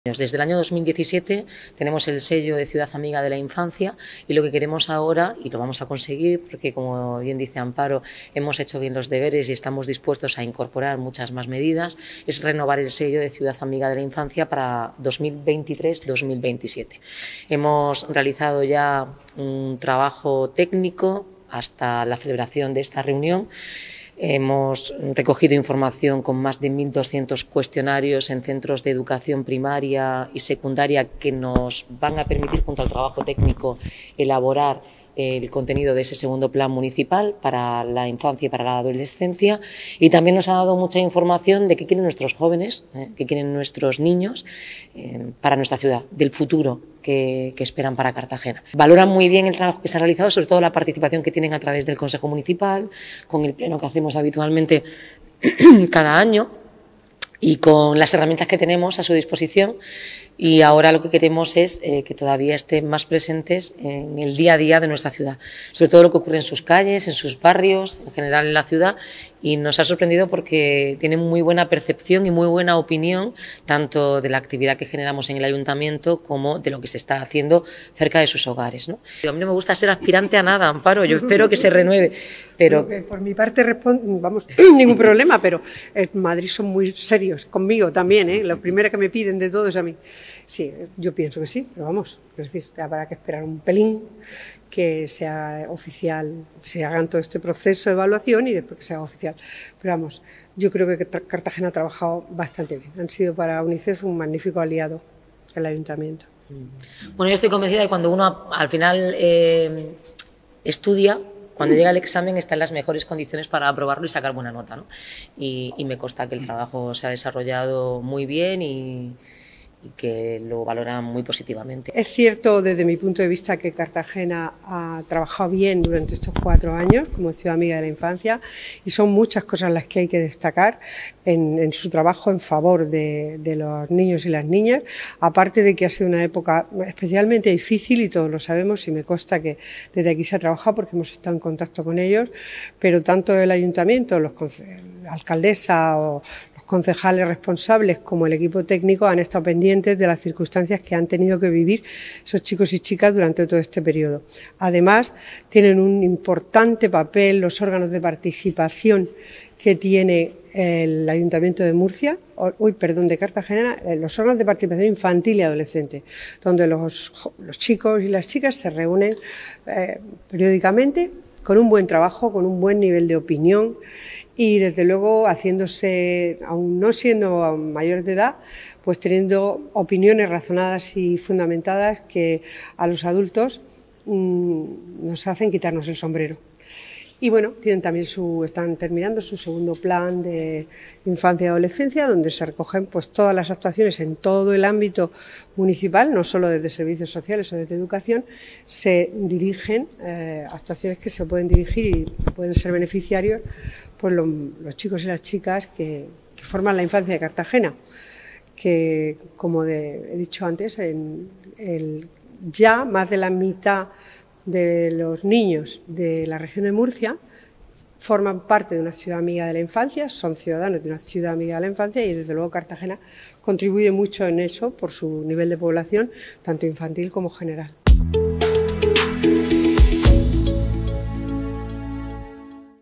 Cartagena revalidará su título como ‘Ciudad Amiga de la Infancia’ de UNICEF con un nuevo plan a...
Cartagena está reconocida por UNICEF como ‘Ciudad Amiga de la Infancia’ desde 2017, un sello que el municipio revalidará con un nuevo Plan Local de Infancia y Adolescencia (PLIA) para los próximos cuatro años. El documento recogerá en su desarrollo las acciones llevadas a cabo desde cualquier área del ayuntamiento y que repercuten directa o indirectamente en la vida de los niños y adolescentes para garantizar su bienestar.

La alcaldesa, Noelia Arroyo, se ha reunido hoy con los responsables de UNICEF en la Región de Murcia, encabezados por su presidenta, Amparo Marzal, para analizar el proceso de renovación y valorar los resultados del anterior Plan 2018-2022. Esta reunión es la primera de una amplia agenda de trabajo que la delegación de UNICEF va a mantener durante todo el día en Cartagena.

Durante la reunión se ha puesto en valor el amplio proceso de participación para la elaboración del nuevo plan, cuya principal novedad es que “no se ha planteado como un paquete cerrado de acciones, sino que marca unas líneas estratégicas que se desarrollaran en torno a planes anuales de acción. “Es un plan vivo y abierto, con capacidad de adaptación”, ha explicado Arroyo.

El proceso de participación del nuevo PLIA “ha supuesto un salto cualitativo muy importante con respecto al anterior”. Por un lado, se han realizado 1.200 cuestionarios online en centros educativos de primaria y secundaria, y por otro lado se ha contado la opinión de técnicos municipales, entidades sociales, menores, monitores, familias, Policía Local, Protección Civil, etc.

OPINIONES PARA EL DISEÑO DE LA CIUDAD

Noelia Arroyo ha subrayado que toda la información recogida, especialmente la de niños y adolescentes, ha servido para recopilar opiniones que también van a ser muy valiosos en el proceso de diseño de la ciudad. “La opinión de los más jóvenes es muy importante”.

Por ejemplo, la encuesta revela que más del 70% de los encuestados está a gusto con los espacios naturales, cerca de un 70% opinan que es una ciudad cómoda para vivir, el 67% valoran las tiendas y el 62% la oferta de ocio.

El 42,6% de encuestados se identifica especialmente con su barrio, un 30% con toda Cartagena y un 27,4% se siente identificado más particularmente con su calle.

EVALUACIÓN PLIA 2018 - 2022

Uno de los aspectos más importantes de lo logrado durante estos últimos cuatro años ha sido que la estructura de participación se ha afianzado. En concreto, la evaluación recoge expresamente el compromiso con la Participación Infantil mediante el apoyo estable y continuado al funcionamiento del Consejo de Infancia y Adolescencia de Cartagena, así como las acciones para dar visibilidad a los Derechos de la Infancia.

De ahí, que el nuevo PLIA proponga la creación de una Comisión de Seguimiento y Coordinación y potenciar los cauces de participación como el pleno, que cada año se celebra el 20 de noviembre para dar la voz a los niños del municipio.